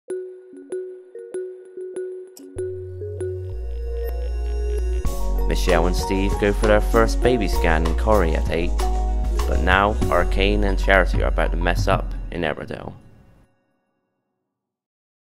Channel 9 ident
This is the 15 second ident that was tasked of creating in my lvl 3 media course.